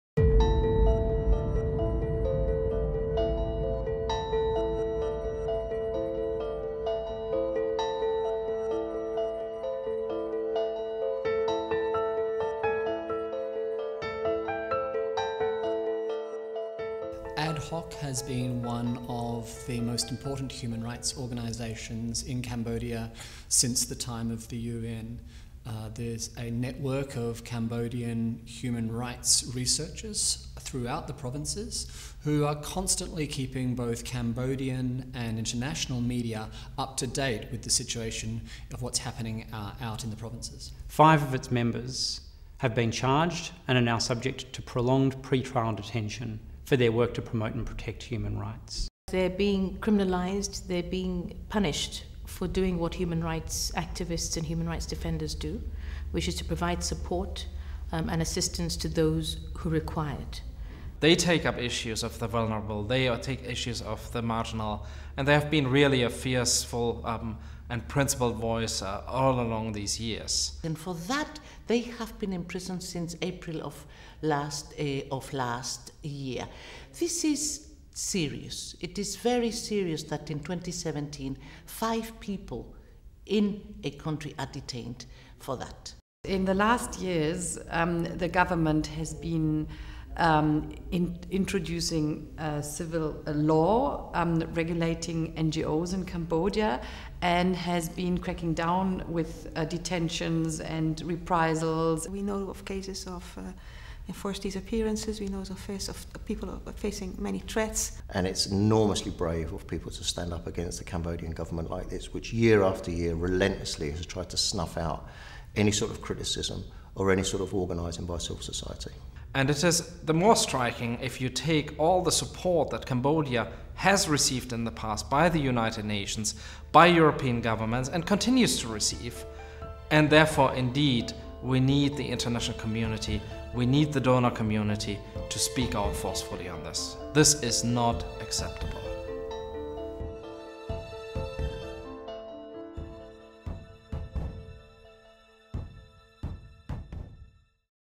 The Khmer 5 - Cambodia - Martin Ennals Award Finalist 2017
Mr Ny Sokha, Mr Yi Soksan, Mr Nay Vanda, Ms Lim Mony and Mr Ny Chakrya – also known as the “Khmer 5” - are five Cambodian human rights defenders who have been arbitrarily detained since 28 April 2016 as a result of their legitimate human rights work. The five human rights defenders have all been working in the field of human rights their entire lives, and together they have a long history of assisting victims of rights violations. They have taken leading advocacy roles, calling for the promotion and protection of human rights in Cambodia, and worked to empower thousands of Cambodians to actively defend their rights.